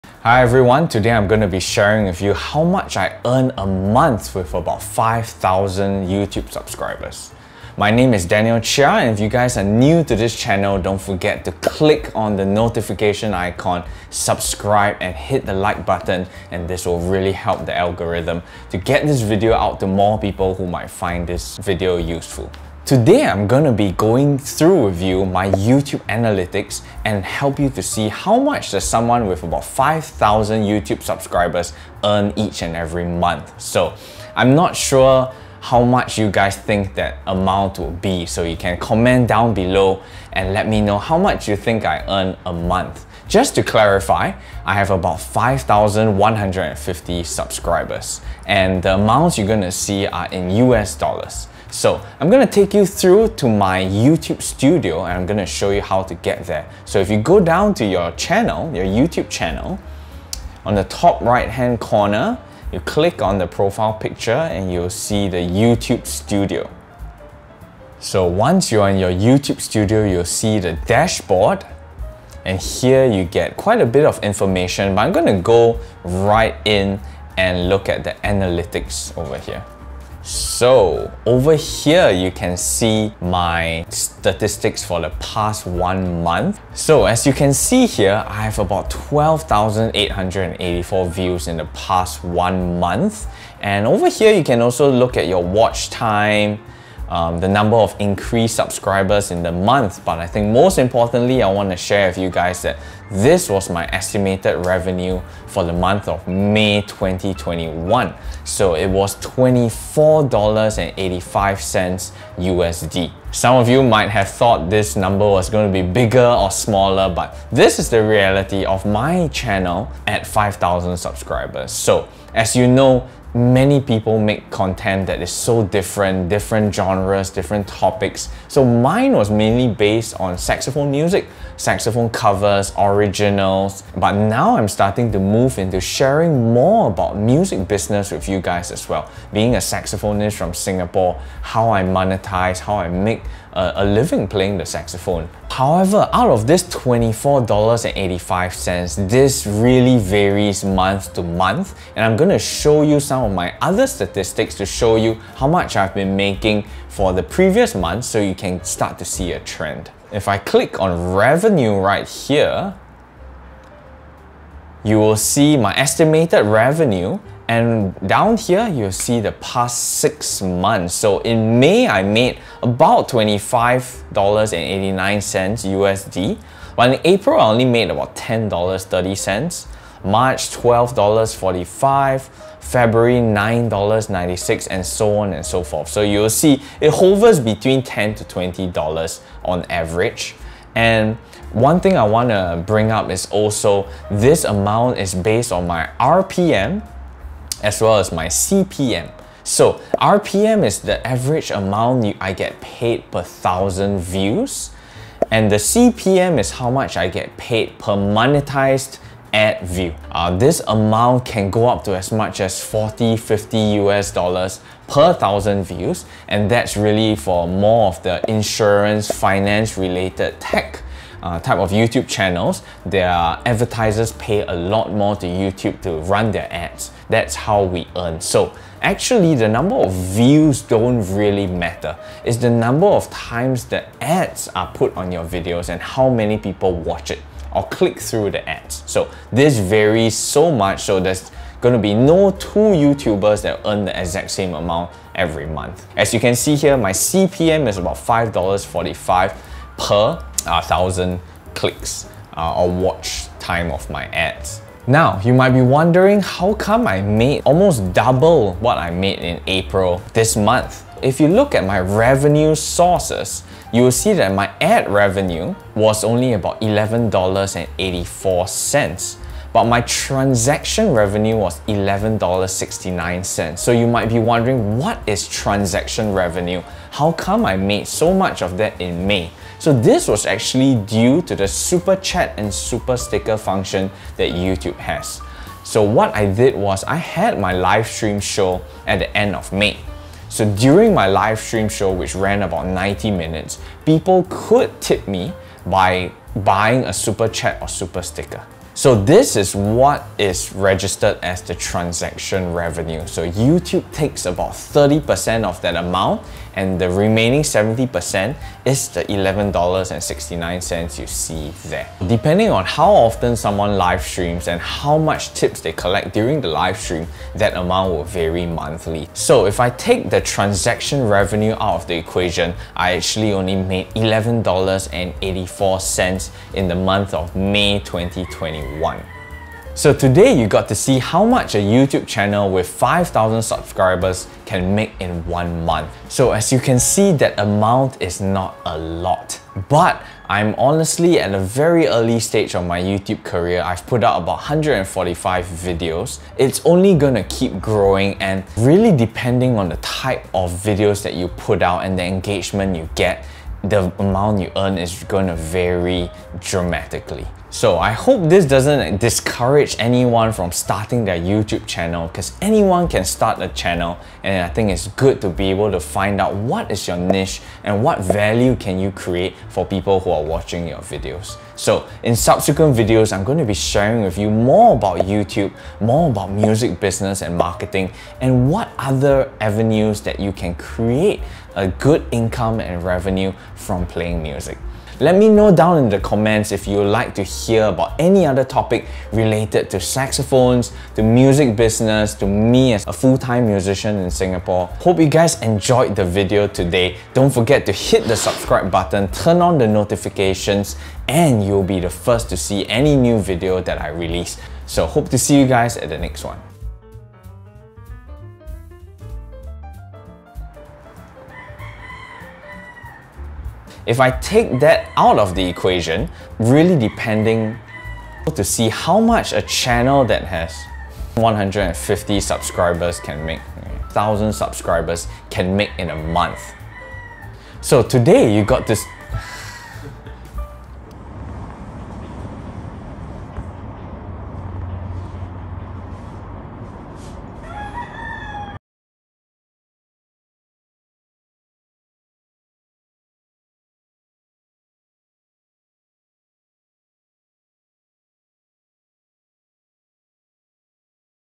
How Much YouTube Paid Me For 5000 Subscribers | Saxophone Music Channel
In this video, I will be showing you how much money a small YouTuber of about 5000 subscribers can make as well as other useful metrics to look out for.

Leave your video suggestions down in the comments!